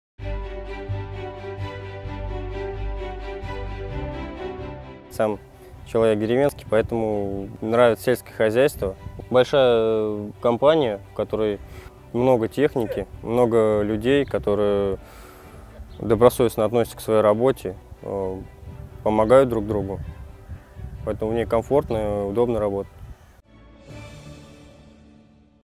«Почему я работаю в АгроТерре?» - интервью Сергея Юрьева, тракториста-машиниста
По словам тракториста-машиниста Сергея Юрьева из Богородицкого хозяйства Тульской области, «АгроТерра»  - большая компания, в которой люди добросовестно относятся к своей работе  и всегда готовы прийти на помощь коллегам. Что ещё нравится Сергею в  своей работе, смотрите в нашем еженедельном видеосюжете.

Сергей Юрьев говорит: Я сам человек деревенский, поэтому нравится сельское хозяйство.  Большая компания, в которой много техники. Много людей, которые добросовестно относятся к своей работе, помогают друг другу, поэтому мне комфортно и удобно работать.